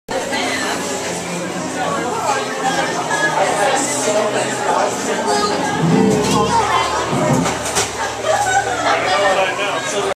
Butter Beer in the Hog's Head Pub
The Hog's Head is another pub at The Wizarding World of Harry Potter.
You can see one of the barmaids adding the head to a butter beer